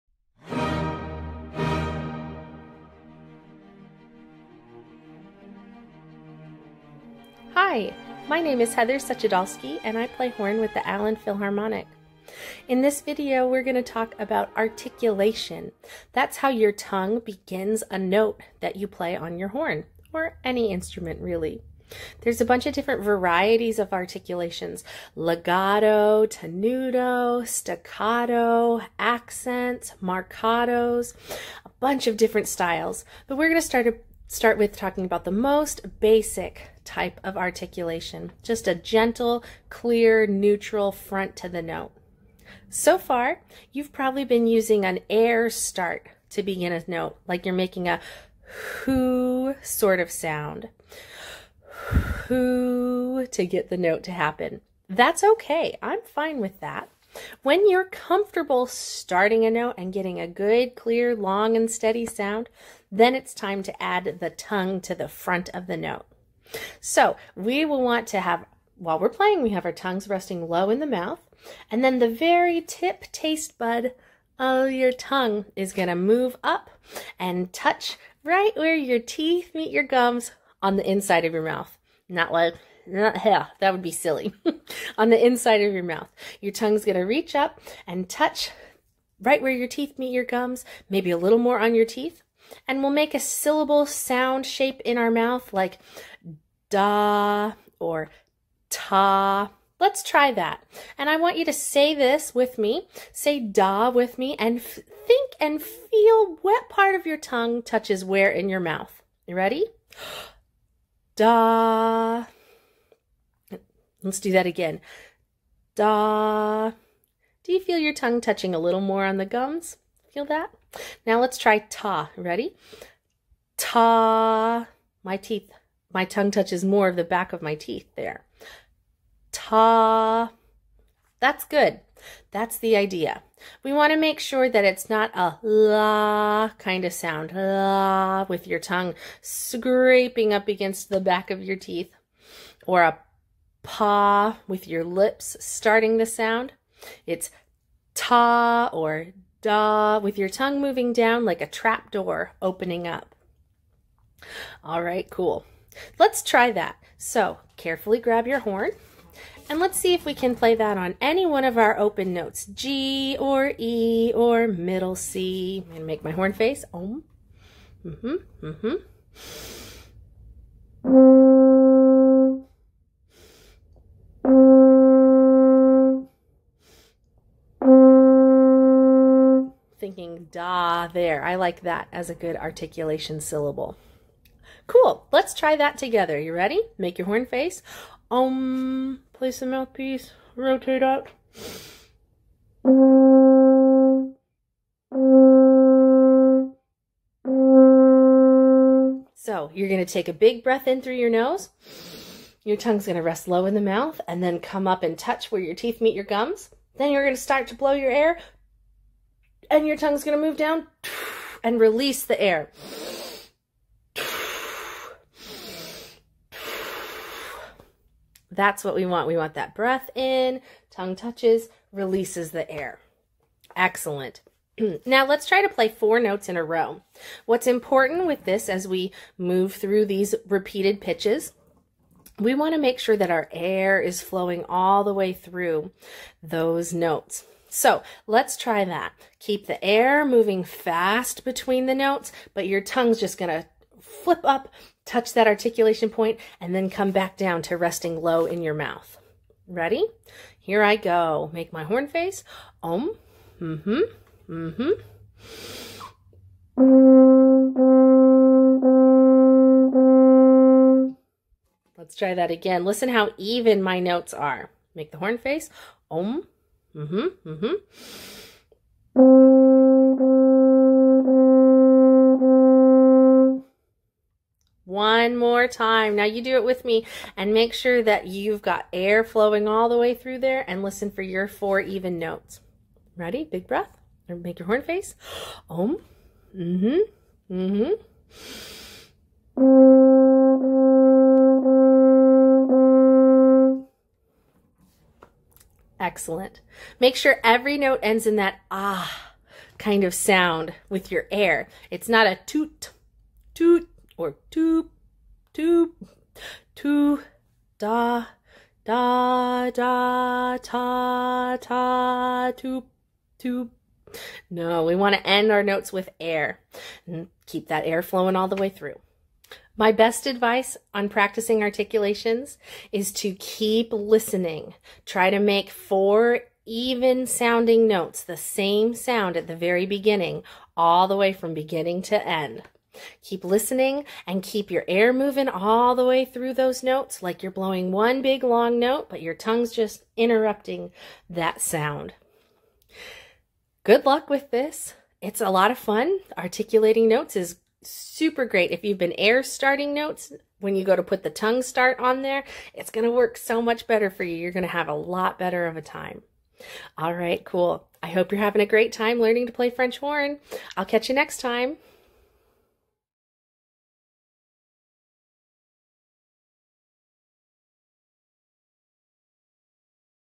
French Horn - Articulation Development
Presented by Heather Suchodolski, Horn player in the Allen (TX) Philharmonic Orchestra.

NOTE:  This video is mirrored.